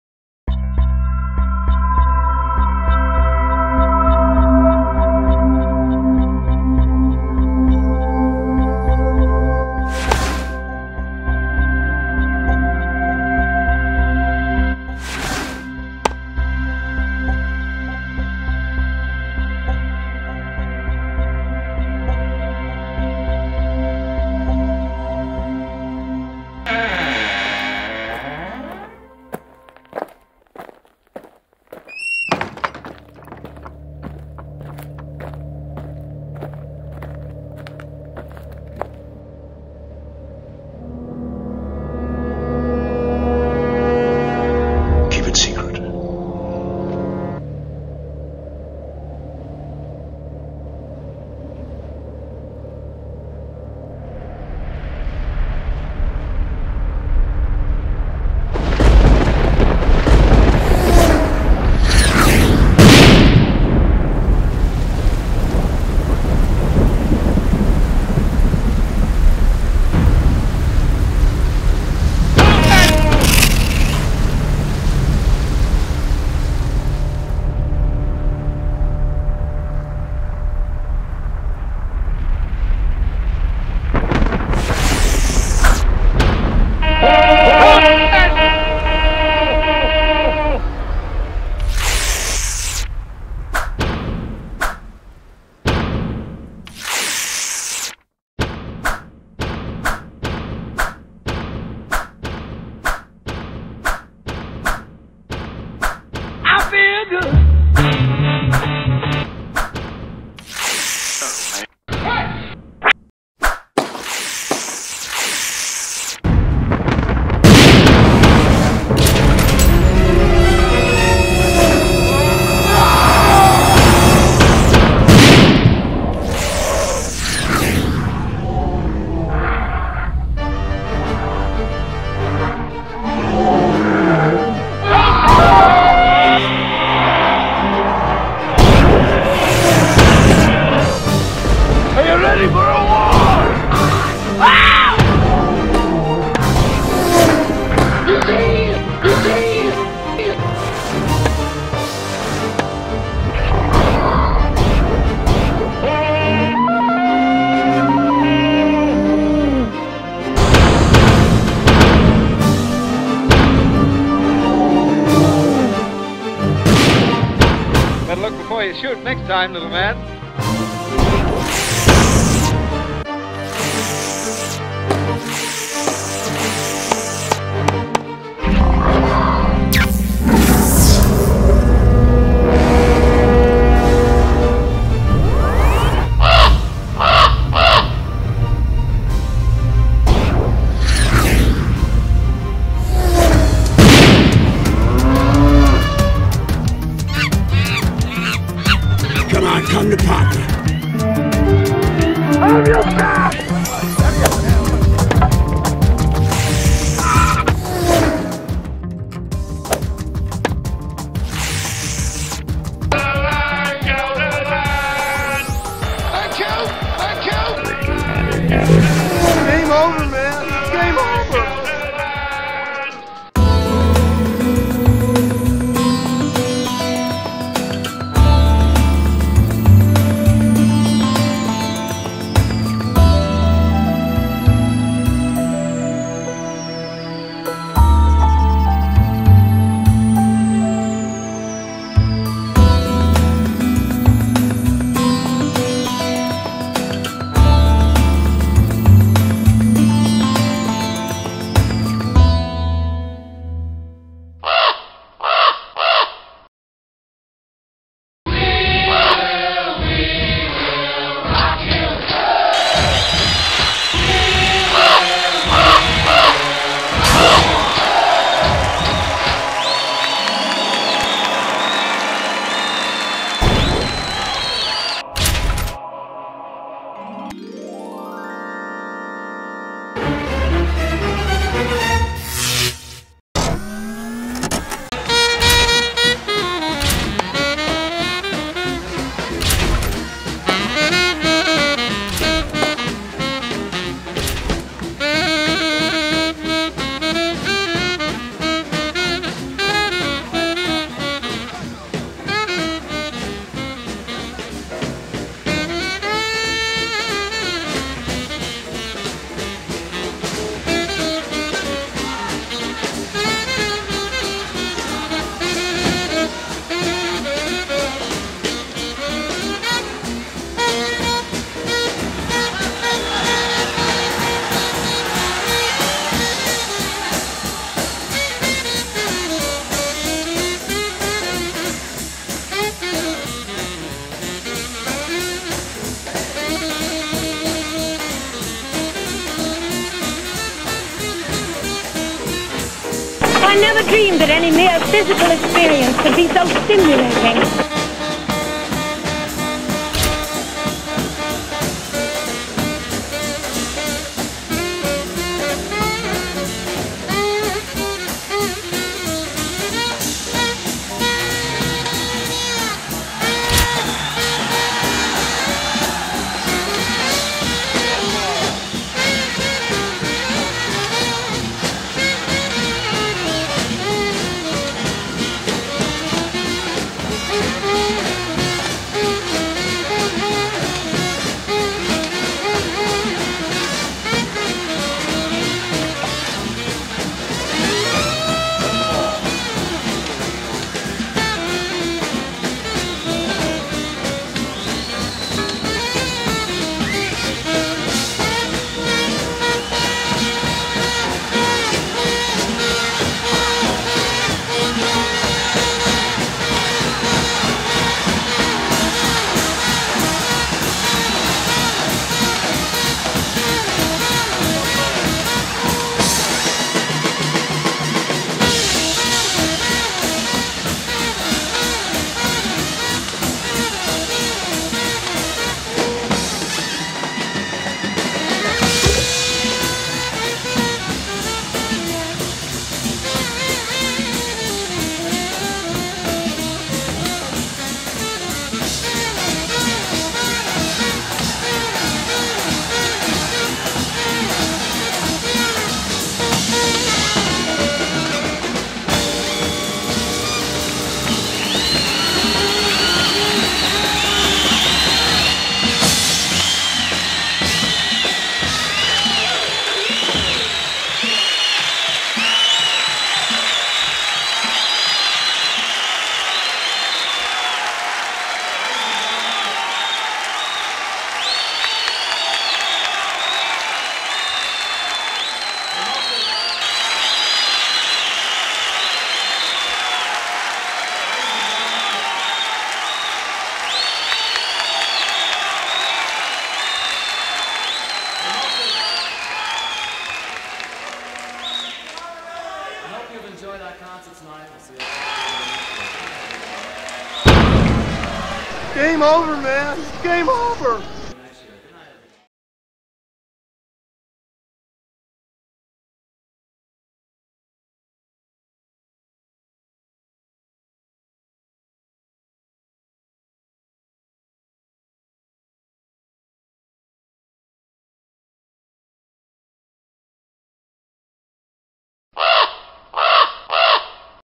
Airgames&Semagria
Director's Cut: An alternative 'Game/Sport' that avoids, or greatly mitigates, injuries due to it's play anywhere attributes which keeps you away from hard concrete courts and uses the natural symmetrical physiology of the human body to perform an aerobic, healthful series of movements as opposed to concentrating high impact strain on any one particular joint, muscle, tendon or limb.